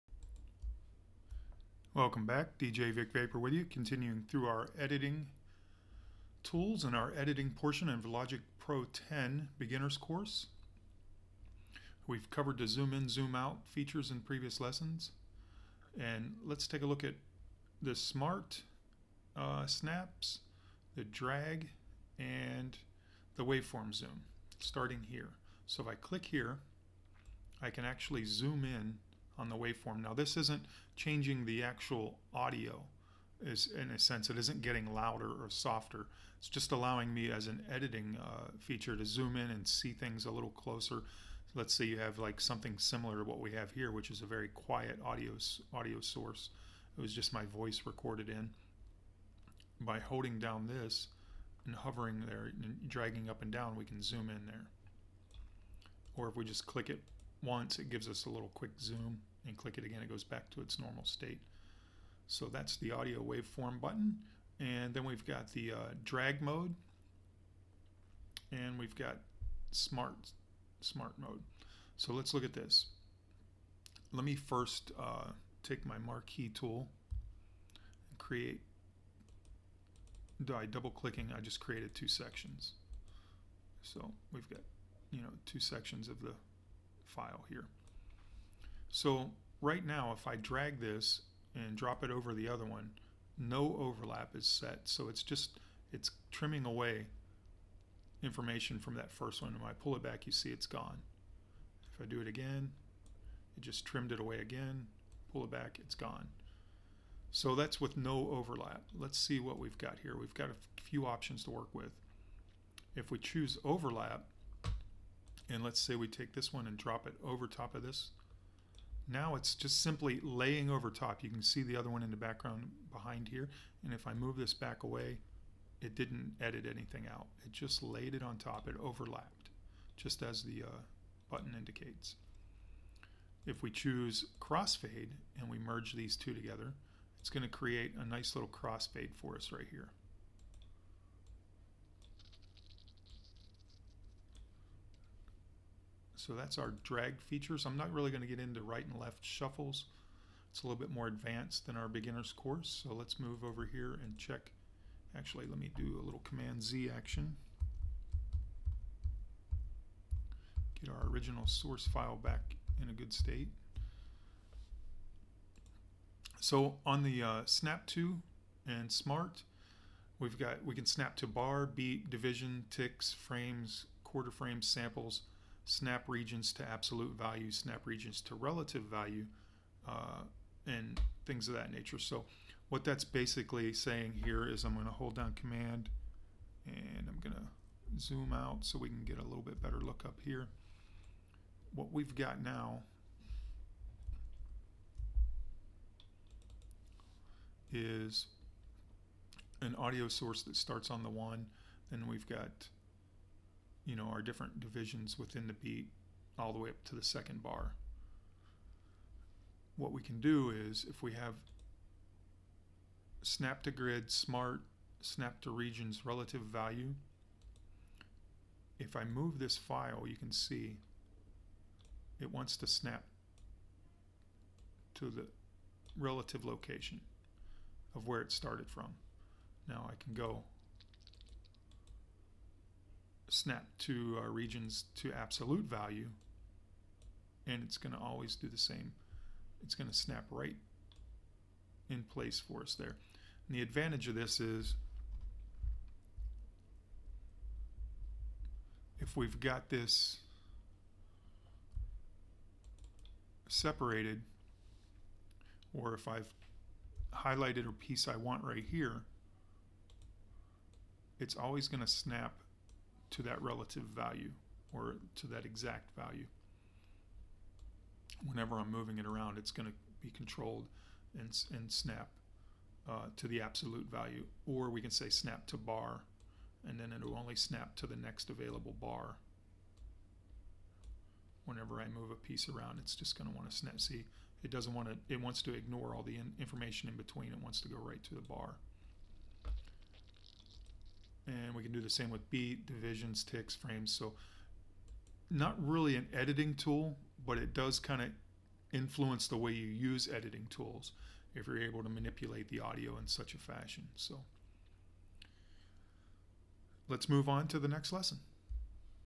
Logic Pro X - Beginners Course - Lesson 21 - Editing Tools Pt. 4 - feat. (djvicvapor)
This course is designed to completely unveil Logic Pro X and get you producing music quickly.  Artist djvicvapor will help you get familiar and comfortable with all of Logic’s amazing features and functions.

If you are a newbie, or an experienced producer switching over from another platform, this course will explain all the essential things you need to know about the basic workings of Logic Pro X. This Beginner's Course will help you build the strong foundation that you need to understand Logic Pro X.